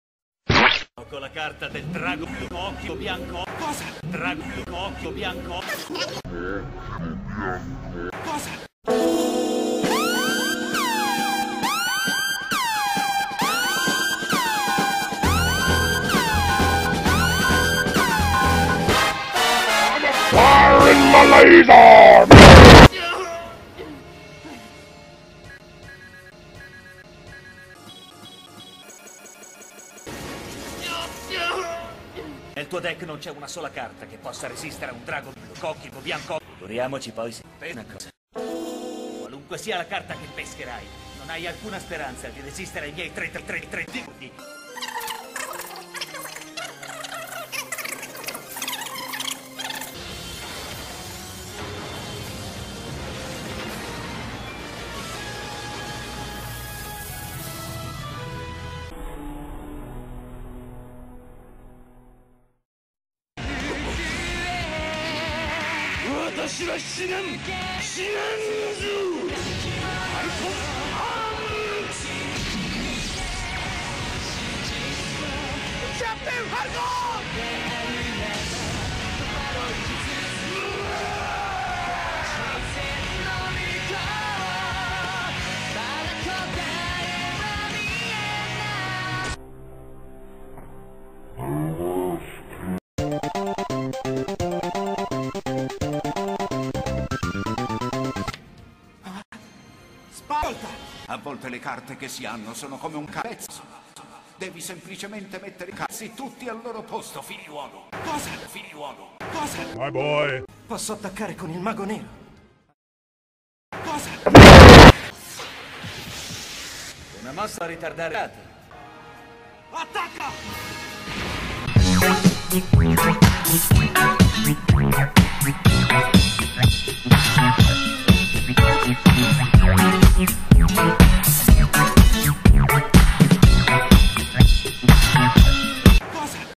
YTP ITA:Il faraone sordo e il torneo di briscola
La finale del torneo rionale di briscola è più infiammata che mai, tanto che le diverse entità soprannaturali evocate nel corso della partita causano parecchi danni alle apparecchiature dello stadio.
Riuscirà il nostro eroe a vincere nonostante il suo grave handicap?

Contiene contenuti di:Daft Punk. (Devo dirlo, non vorrei farmi Swappare la mia prima Poop...)